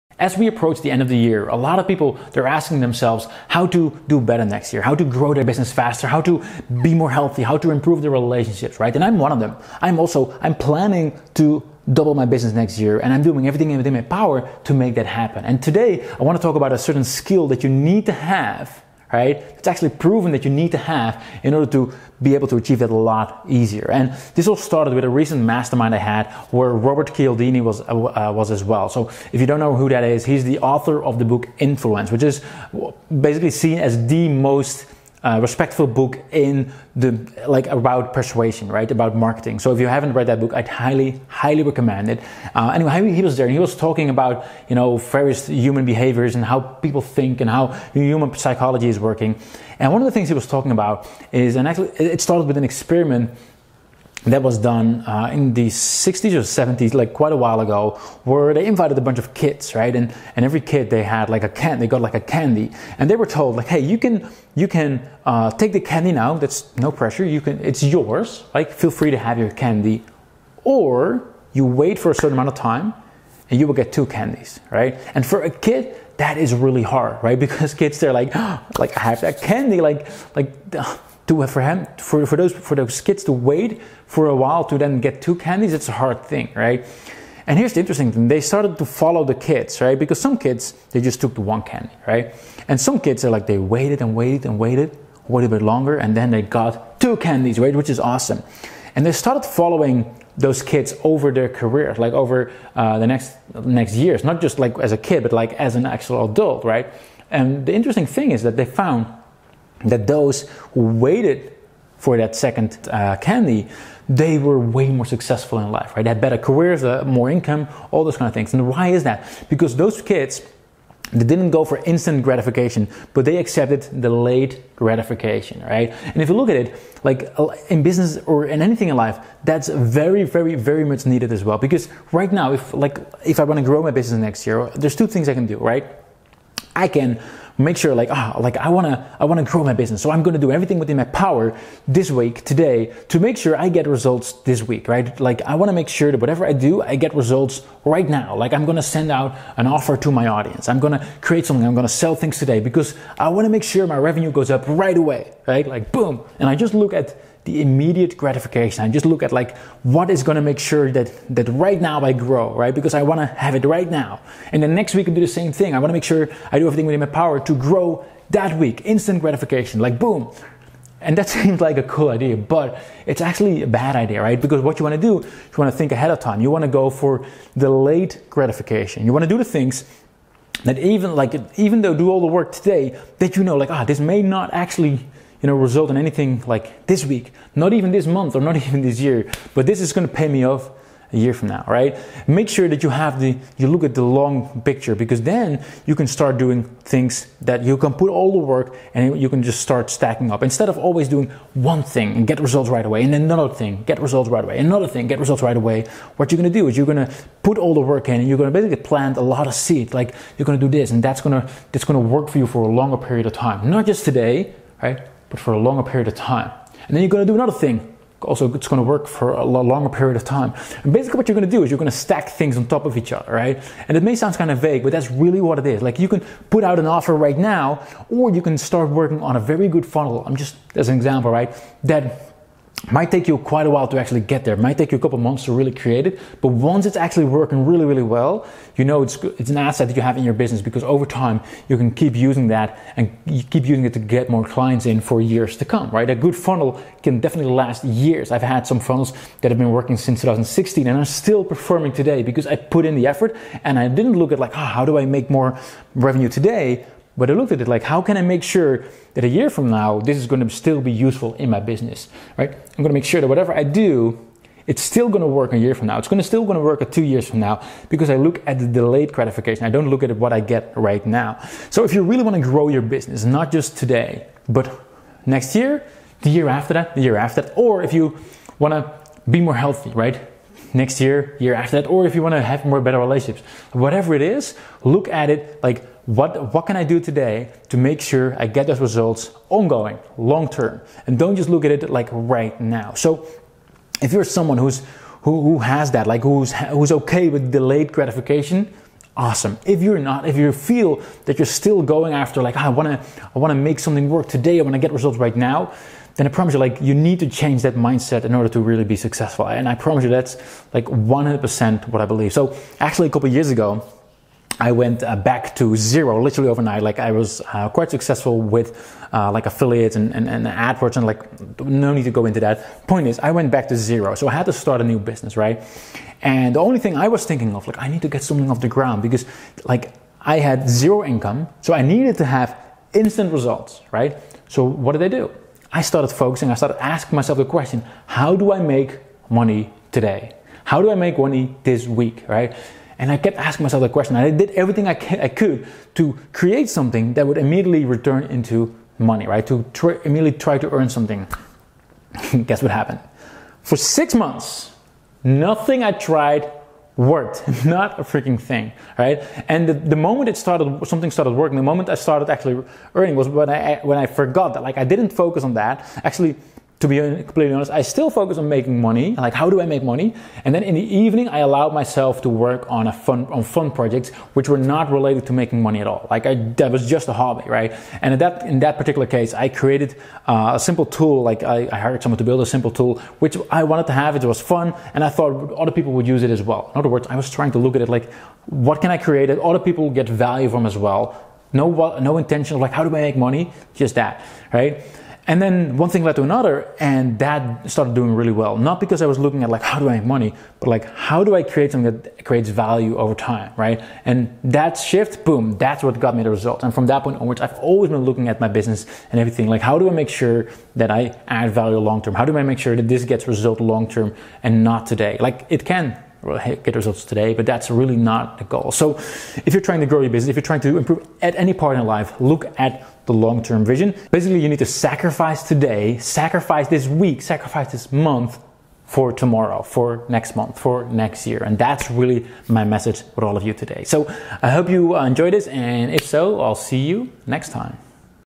How to make next year the BEST EVER using delayed gratification
The New Year is a blank slate to be filled with the dreams and goals you have for your business. I’m sure that no matter what they are, they’re meant to do one thing…

But as excited as you are to implement new strategies, it’s important to consider the big picture. Will they benefit your business just for today or long term?

For that reason, I’d like to talk to you about DELAYED GRATIFICATION. Learn why controlling your impulses can actually help you succeed.